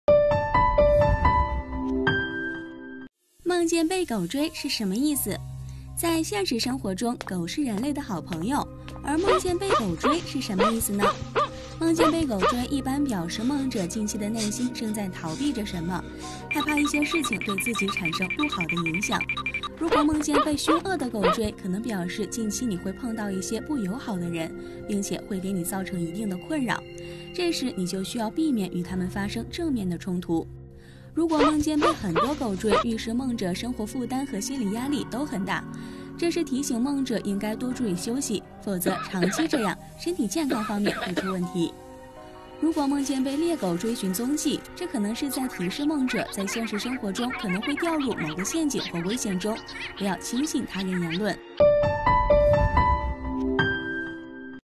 夢見被狗追是什么意思
表示近期夢者會碰到一些不友好的人，并且會給夢者造成一定的困擾； 如果夢見被很多狗追，預示夢者生活負擔和心理壓力都很大；如果夢見被獵狗追尋蹤跡，提示夢者在現實生活中可能會掉入某個陷阱或危險中。